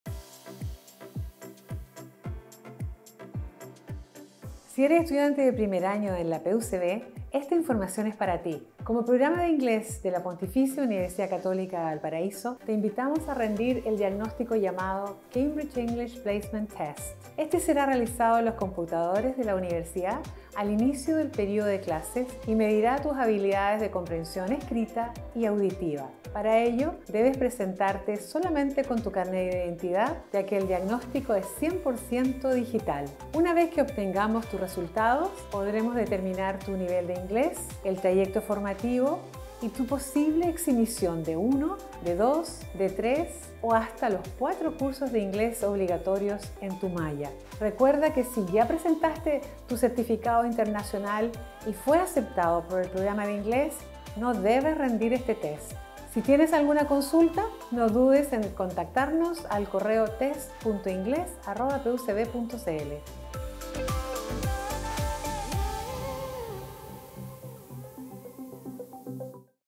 Examen diagnóstico de Inglés en la PUCV
Si eres estudiante de primer año en la Pontificia Universidad Católica de Valparaíso, el Programa de Inglés te invitará a rendir el test diagnóstico llamado Cambridge English Placement Test (CEPT) en los computadores de la universidad al inicio del periodo lectivo para determinar tu nivel de inglés, el trayecto formativo o tu posible eximición de 1, de 2, de 3 o de los 4 cursos de inglés, obligatorios en tu malla.